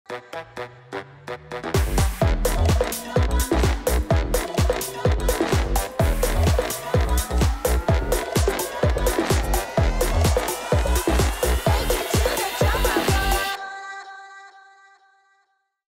Tiki Waikiki Cocktails @ TGI Fridays™
Ζήσε το απόλυτο Tiki Summer στα TGI Fridays™ με τρία νέα πολύχρωμα Tiki Waikiki Cocktails: Havana Wave, Paradise Beach, Pineapple Ginger Splash, αλλά και τα all time tiki favorites. Feel the Tropical Vibes.